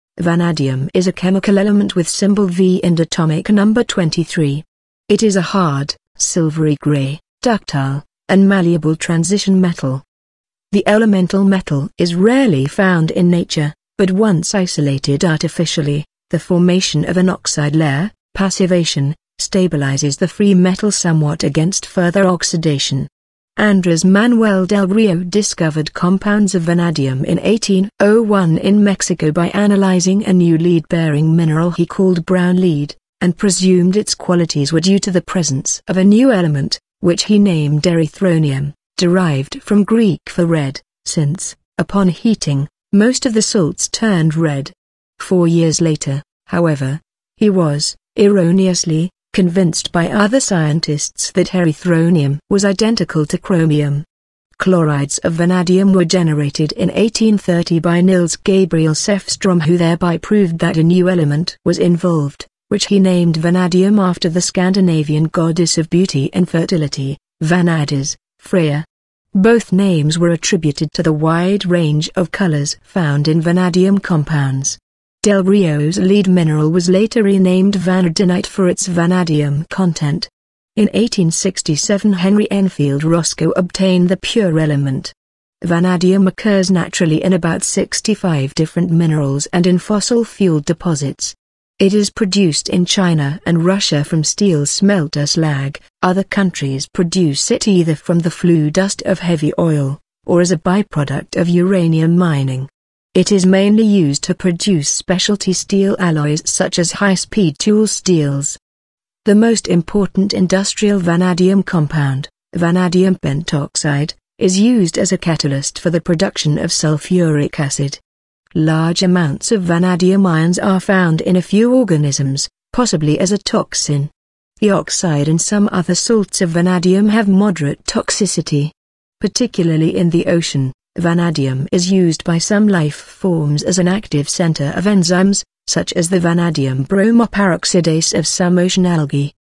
What is Vanadium?
Vanadium is a chemical element with symbol V and atomic number 23. It is a hard, silvery grey, ductile, and malleable transition metal. The elemental metal is rarely found in nature, but once isolated artificially, the formation of an oxide layer (passivation) stabilizes the free metal somewhat against further oxidation.

Andrés Manuel del Río discovered compounds of vanadium in 1801 in Mexico by analyzing a new lead-bearing mineral he called "brown lead", and presumed its qualities were due to the presence of a new element, which he named erythronium (derived from Greek for "red") since, upon heating, most of the salts turned red. Four years later, however, he was (erroneously) convinced by other scientists that erythronium was identical to chromium. Chlorides of vanadium were generated in 1830 by Nils Gabriel Sefström who thereby proved that a new element was involved, which he named "vanadium" after the Scandinavian goddess of beauty and fertility, Vanadís (Freyja). Both names were attributed to the wide range of colors found in vanadium compounds. Del Rio's lead mineral was later renamed vanadinite for its vanadium content. In 1867 Henry Enfield Roscoe obtained the pure element.

Vanadium occurs naturally in about 65 different minerals and in fossil fuel deposits. It is produced in China and Russia from steel smelter slag; other countries produce it either from the flue dust of heavy oil, or as a byproduct of uranium mining. It is mainly used to produce specialty steel alloys such as high-speed tool steels. The most important industrial vanadium compound, vanadium pentoxide, is used as a catalyst for the production of sulfuric acid.

Large amounts of vanadium ions are found in a few organisms, possibly as a toxin. The oxide and some other salts of vanadium have moderate toxicity. Particularly in the ocean, vanadium is used by some life forms as an active center of enzymes, such as the vanadium bromoperoxidase of some ocean algae.